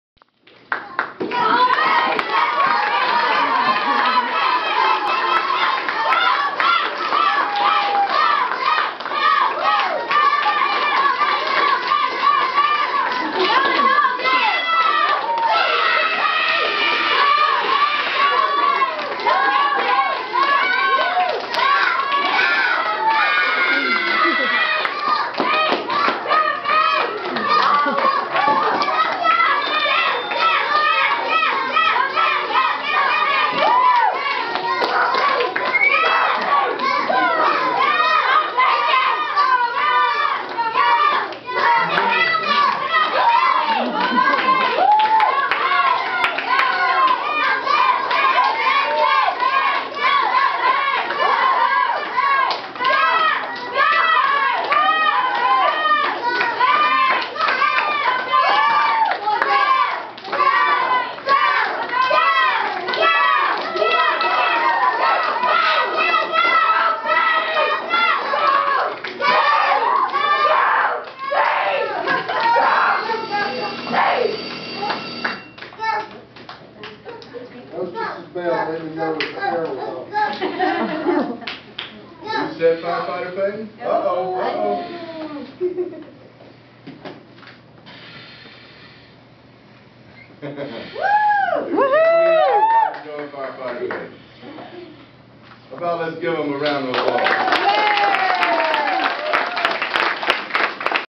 LSU FETI Fieldtrip 2-minute drill
2 minute drill at LSU FETI from Bayou Cajun Homeschoolers Fieldtrip 2/2/12